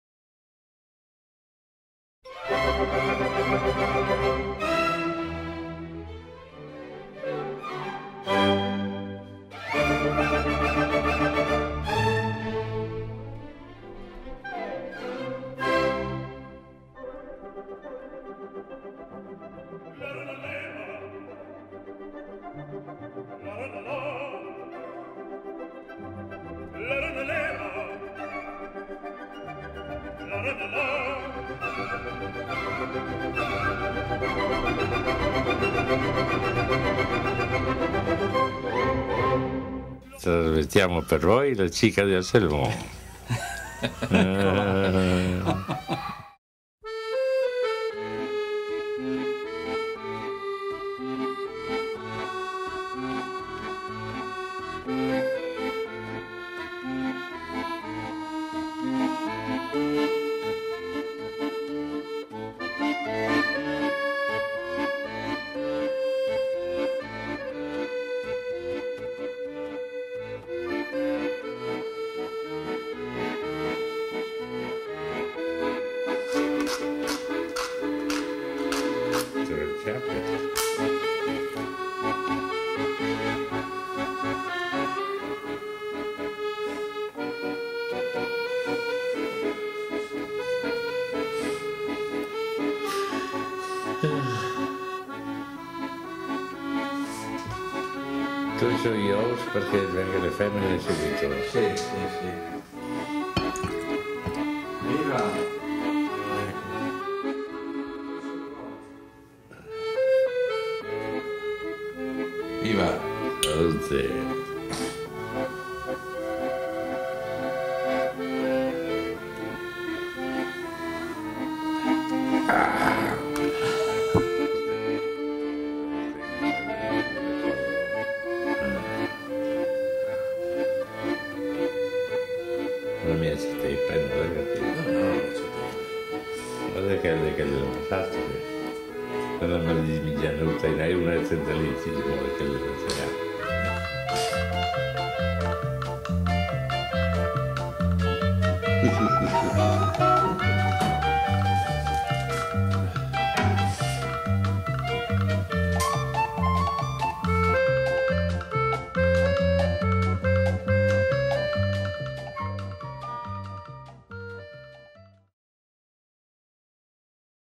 AVIANO LA BARBA DI ANSELMO
Il nostro caro Anselmo dopo il grande successo della famosa "cica di Anselmo" ora in "La barba di Anselmo" mentre si rade la barba e poi naturalmente...rullarsi la famosa cica. Location Marsure di Aviano - (PN) FVG Maggio 2013. Protagonista Tassan Solet Anselmo - Regia e video: Italo Paties Montagner - Videoreporter per "passione".